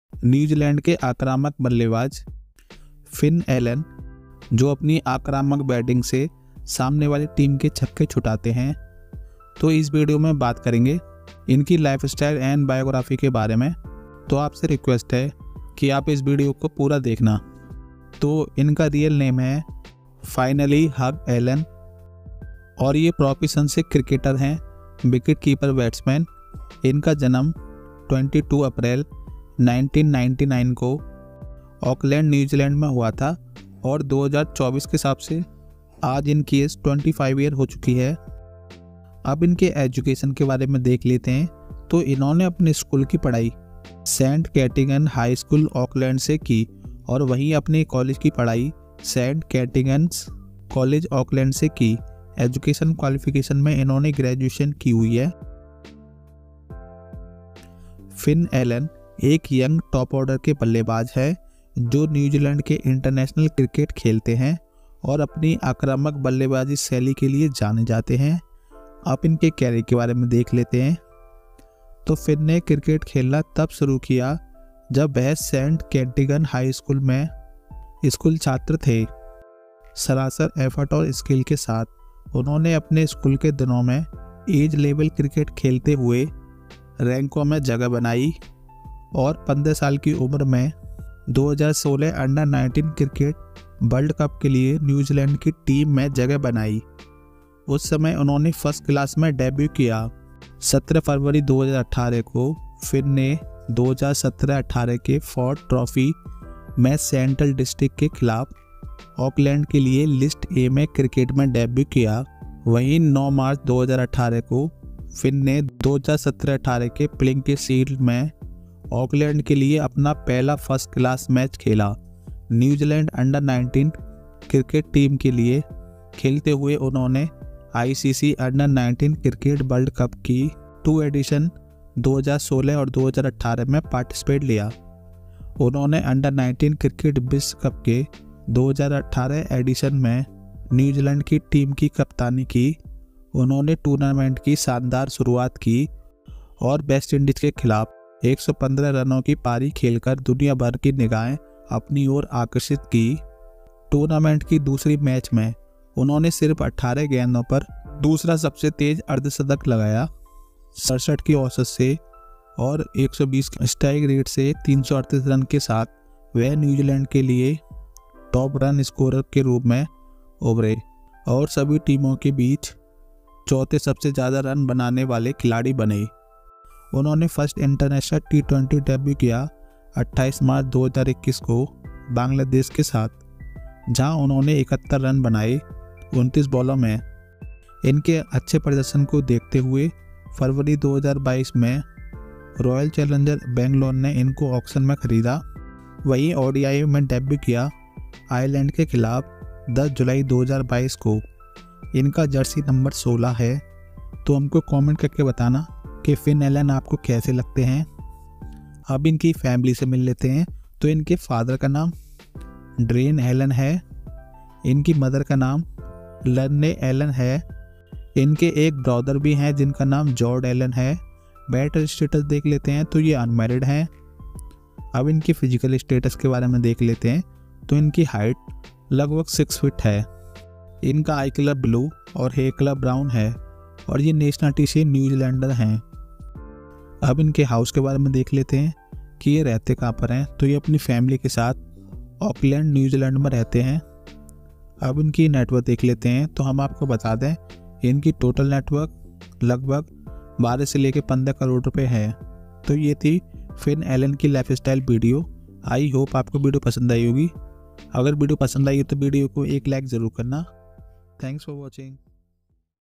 Finn Allen Lifestyle 2024, Age, Family, Biography, Girlfriend, Net worth
Video About:
Hello dosto aaj ki is video mein hum baat krne bale hai finn allen ki lifestyle ke baare mein

Timestamp:
0:00 finn allen lifestyle
0:27 finn allen age
1:09 finn allen career
3:29 finn allen family
3:56 finn allen house
4:05 finn allen net worth